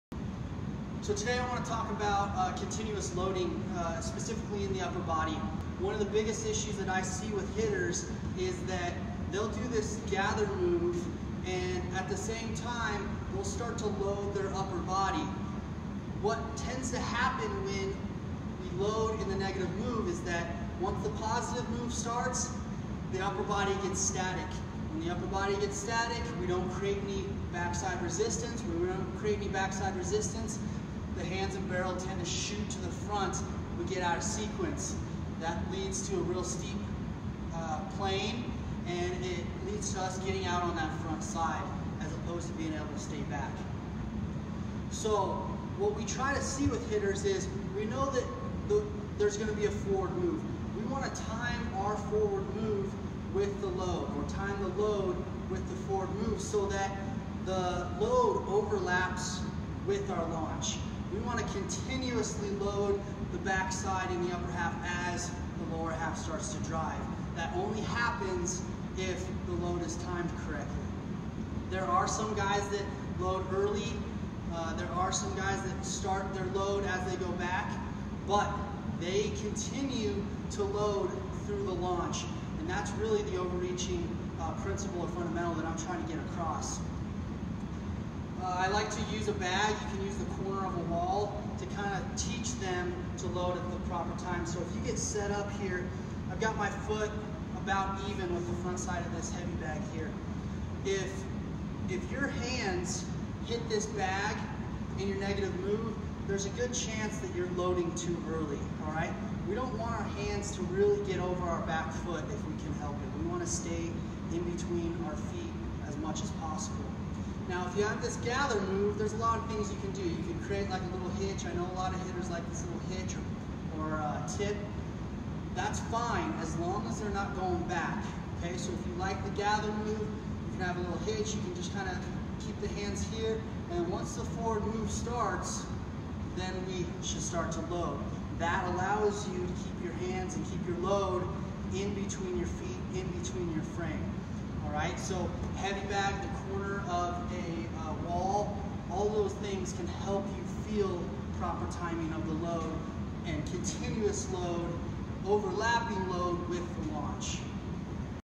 Timing the Load Forward in the Baseball Swing
It is important that your load happens at the correct time to ensure it overlaps properly with the launch. You want to avoid loading to early as it can cause the upper half to get static. When the upper body and barrel get static there is difficult to sequence properly and create good swing plane. It is detrimental to your rhythm and ability to get on time with the pitch.